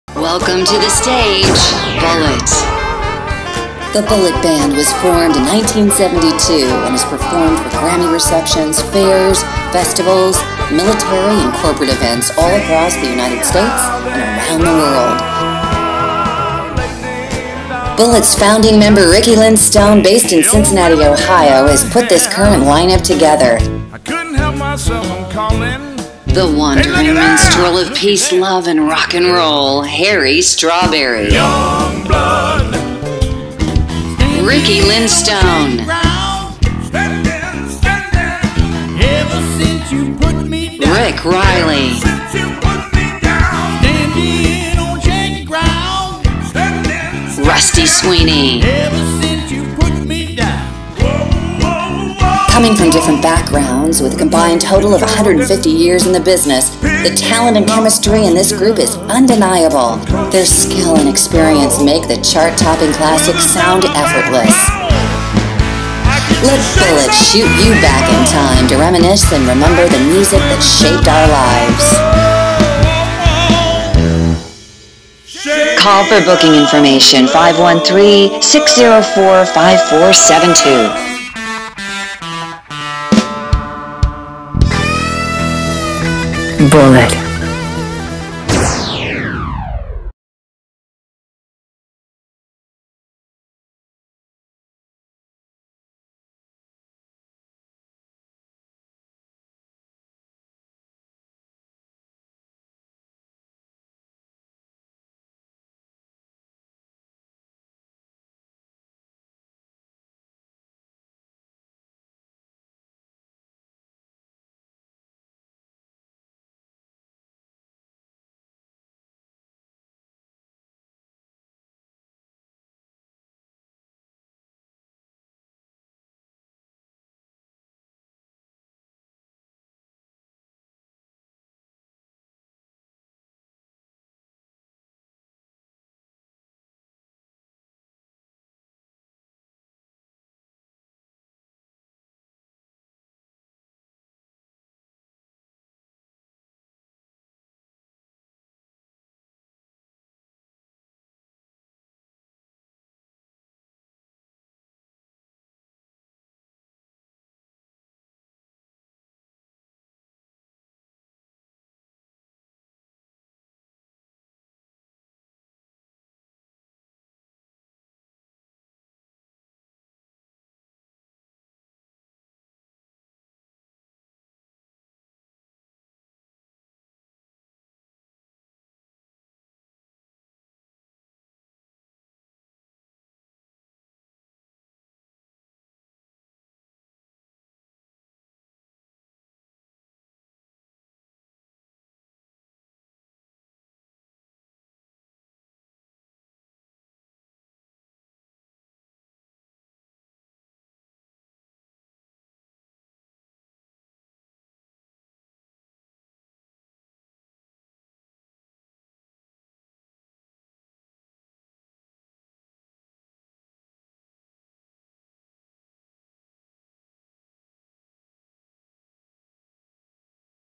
BULLETTPROMOTIONALREEL2017
The Bullett Band was formed in 1972 and has performed for Grammy Receptions, Fairs, Festivals, Military and Corporate Events all across the United States and around the world.  Bullett’s founding member, Ricky Lynn Stone, based in Cincinnati, OH, has put this current line up together while working on original music to be featured on their album due out in the spring of 2017. Coming from different backgrounds with a combined total of over 150 years in the business, the talent and chemistry in this group is undeniable. Their skill and experience make the chart topping classics sound effortless.